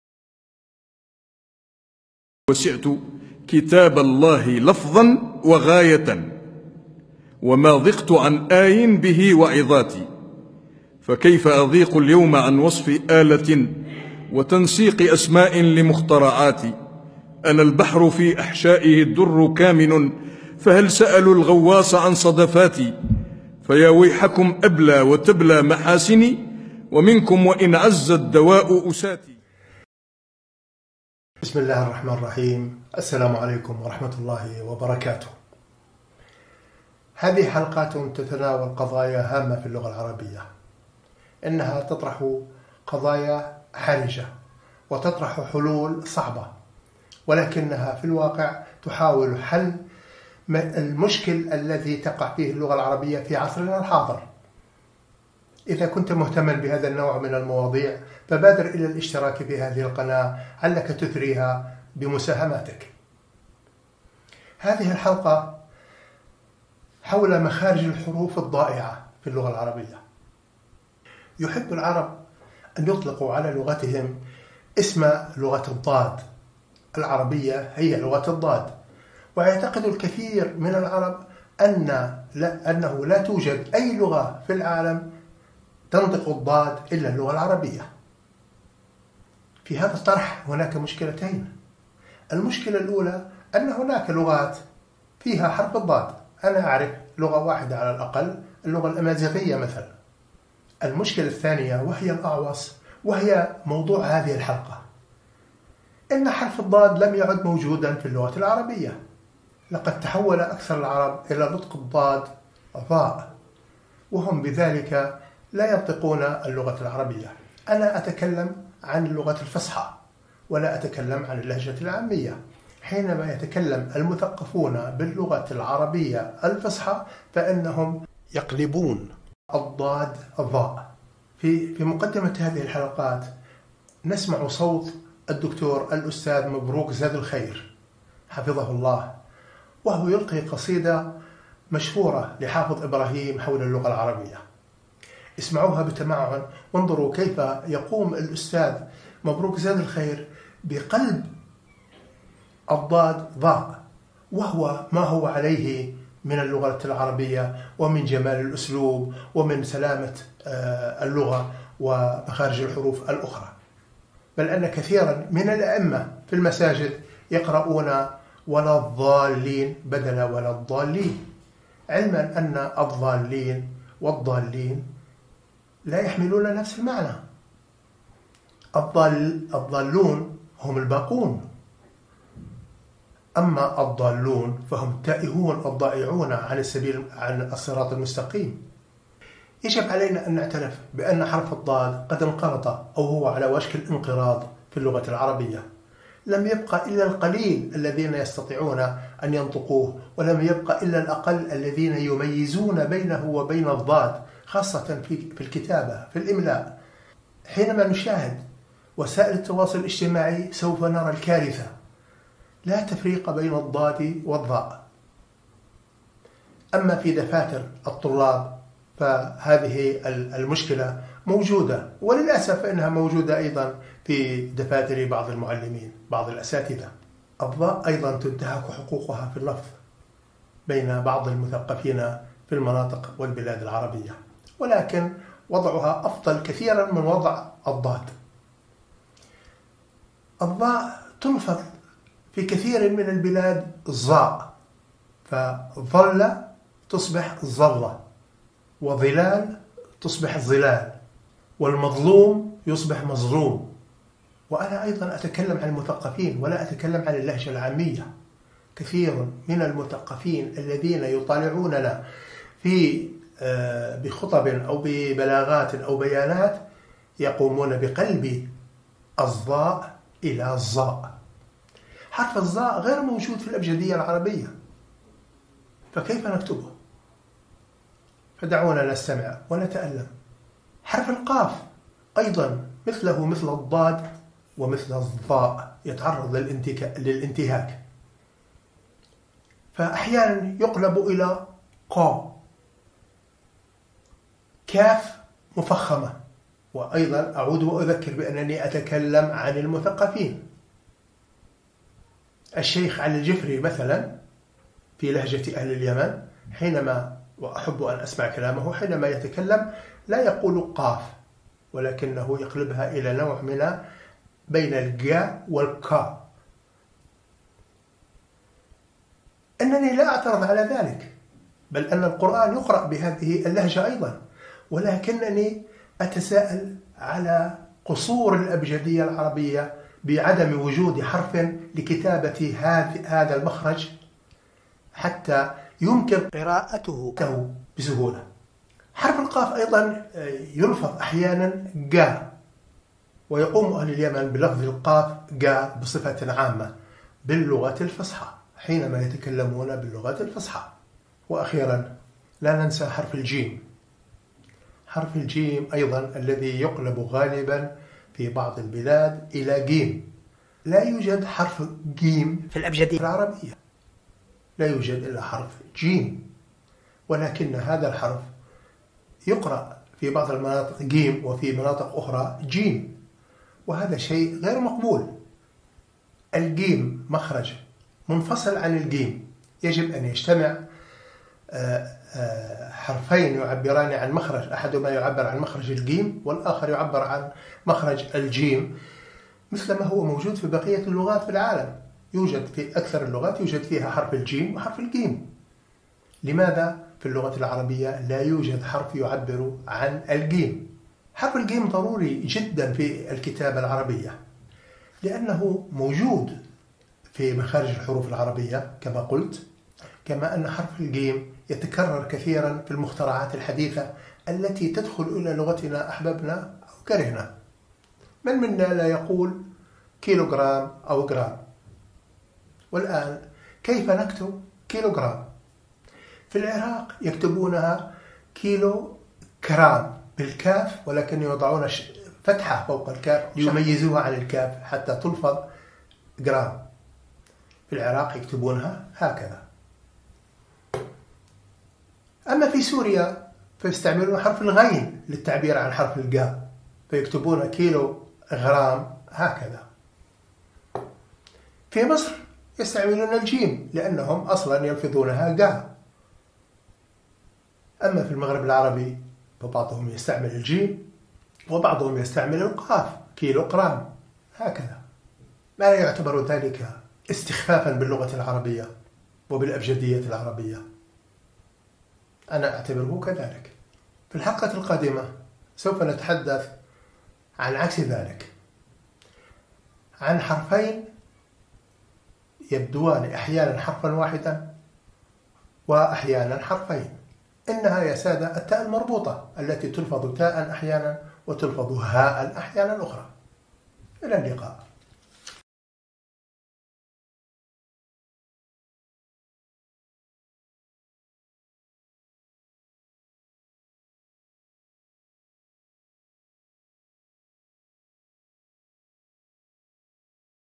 نظرات على اللغة العربية، الحلقة 9 - مخارج الحروف المفقودة
كيف نكتب كيلو جرام في البلاد العربية، انها تكتب بشكل مختلف في كل دولة، والسبب في ذلك لان اعلب العرب ينطقون  العرب الحروف بشكل مختلف عما هي عليه فعلا، فحرف الضاد مثلا وهو رمز من رموز اللغة كالعربية التي تسمى لغة الضاد يلفظ من اغلب العرب ظاء، حتى لو كانوا يتكلمون بالفصحى، ومثله عدة احرف تنطق بغير ما يجب ان تنطق به، مثل حروف الظاء، والقاف، والجيم. مع ان الابجدية العربية لا يوجد بها حروف تغطي اللفظ الجديد.